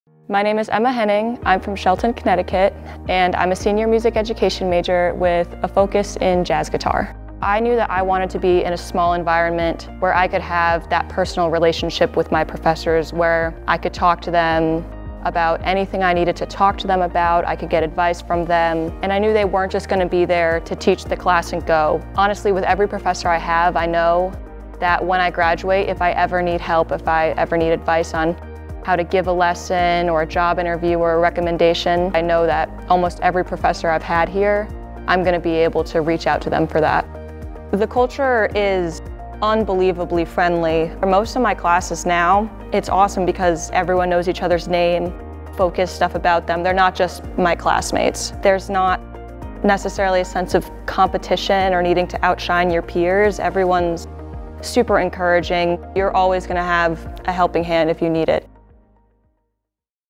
WCSU Music Student Testimonial: BS in Music Education - Emma
WCSU Music major, Emma Henning, explains why the Music Department at Western Connecticut State University was the best choice for her.